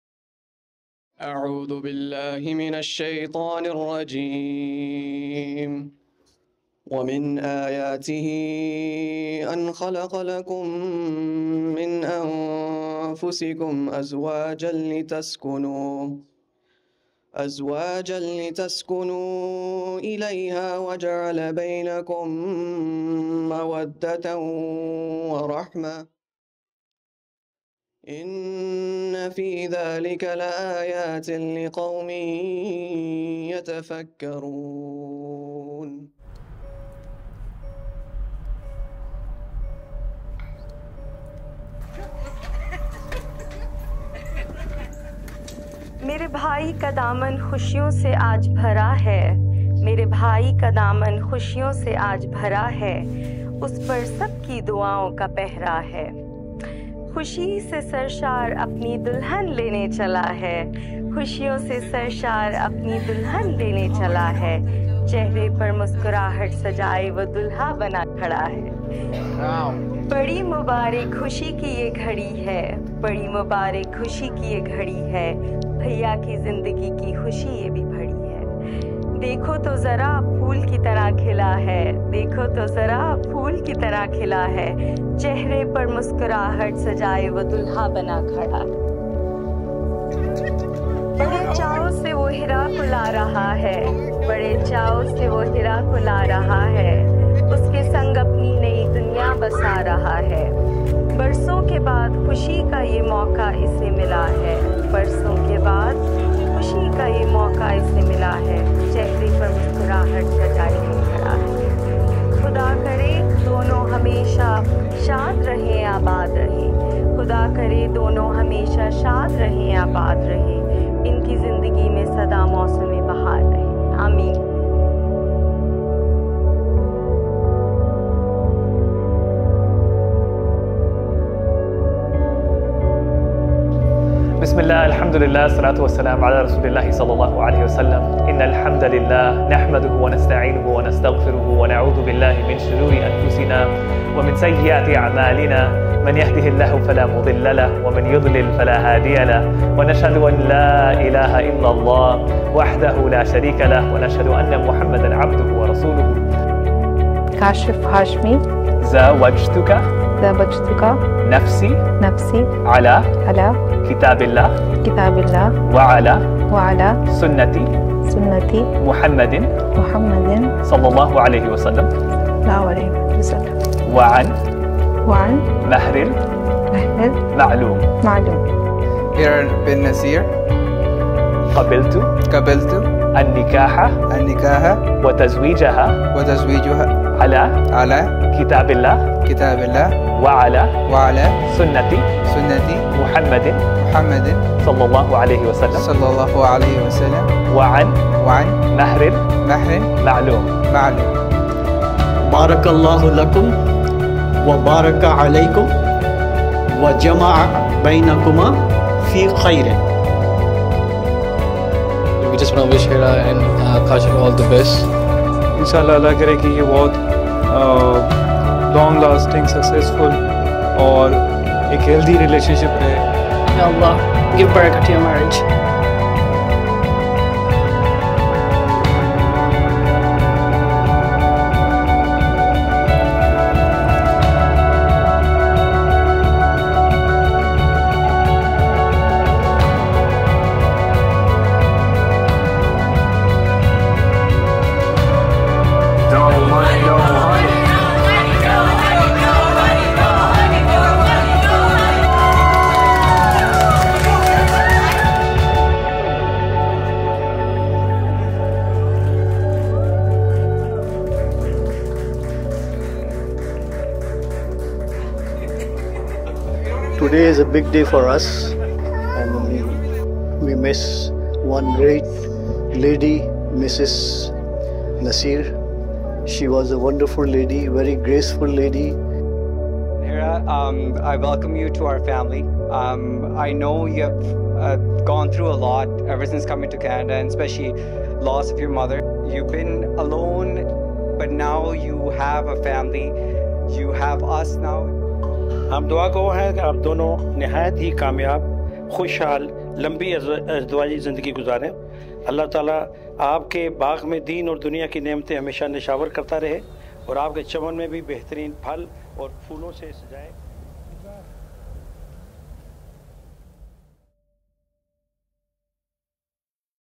Muslim Pakistani Nikkah Wedding Film | Princeton, NJ | Muslim Center of Greater Princeton (MCGP)
Heartfelt, intimate wedding film